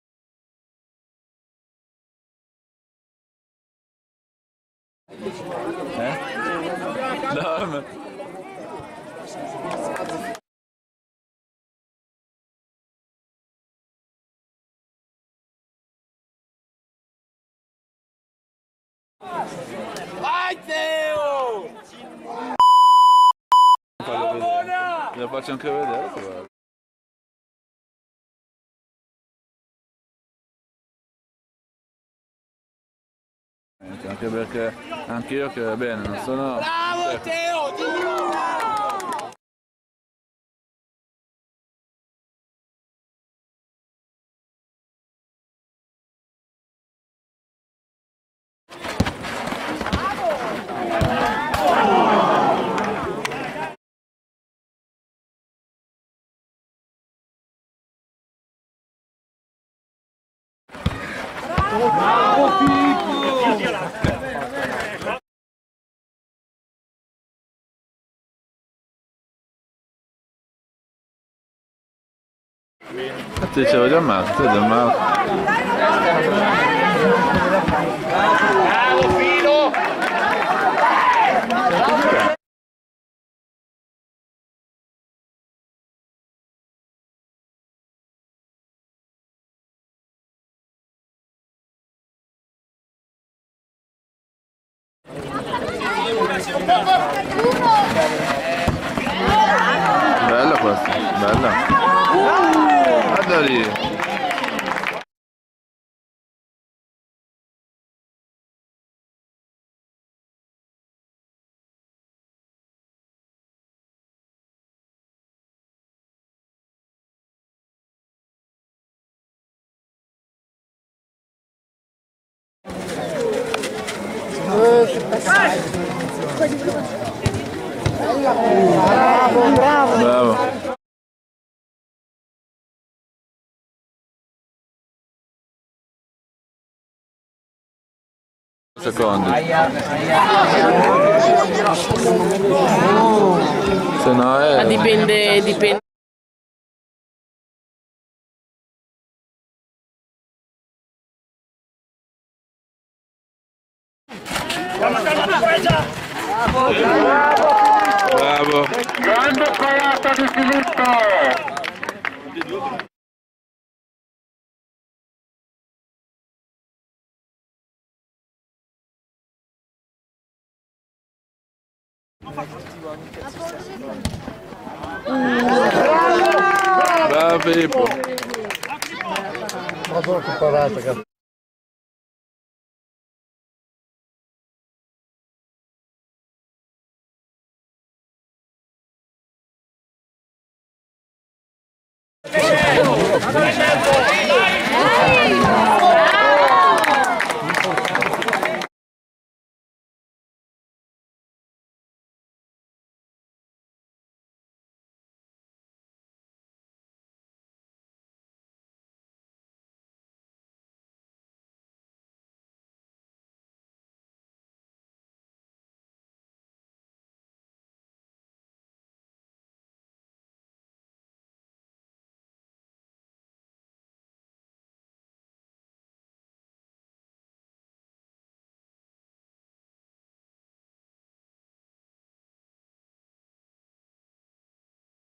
Parate Filippo - Torneo Spiazzi - Estate 2014 (No Musica)
Montaggio Parate Filippo - Torneo Spiazzi - Calcio a 7 - Estate 2014 (No Musica).